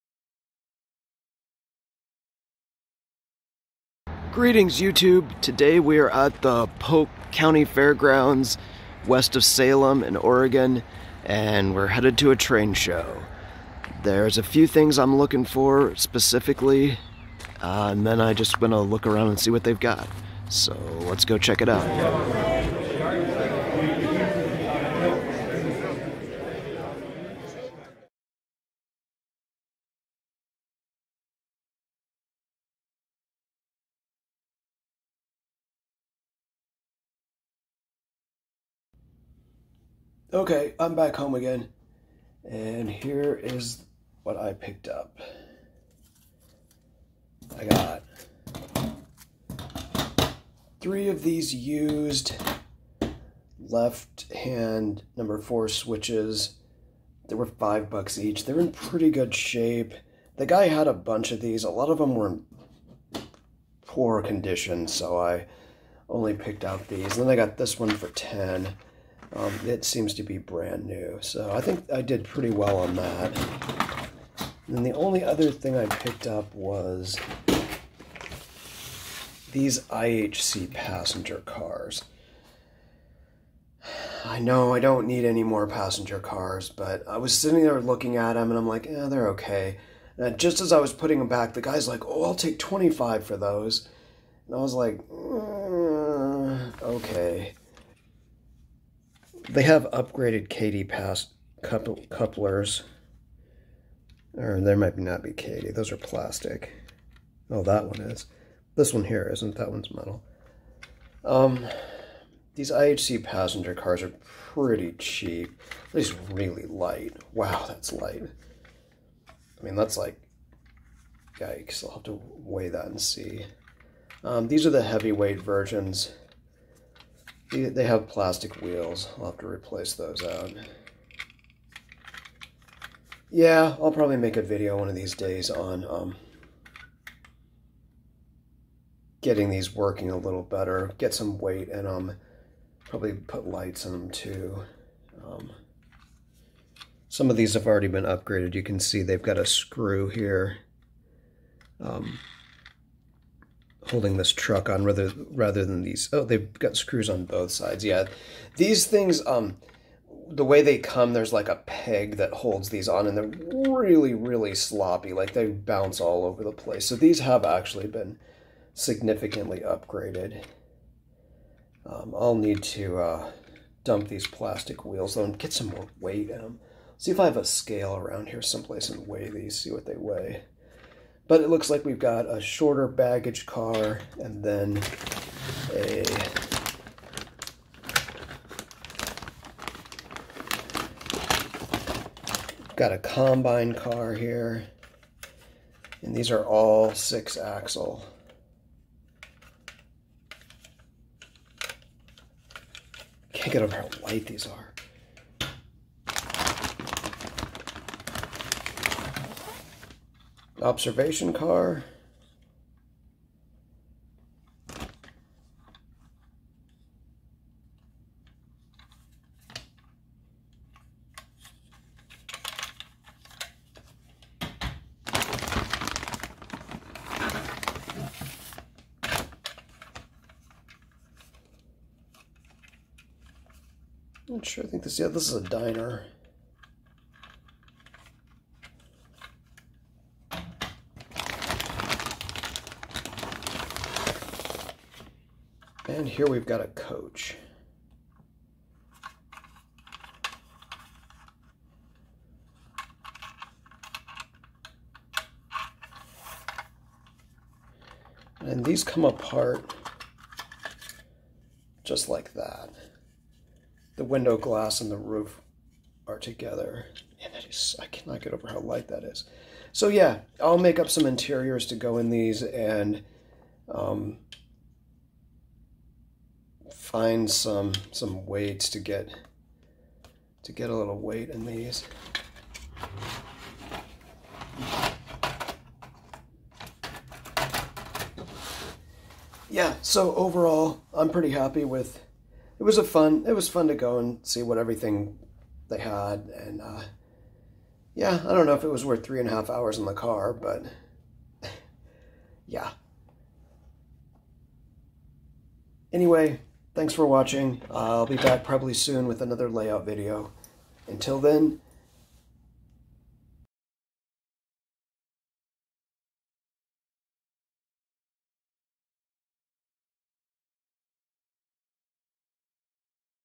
2nd Dec 2023 Swap Meet
Paying a visit to the swap meet at the Polk County Fairgrounds west of Salem Oregon.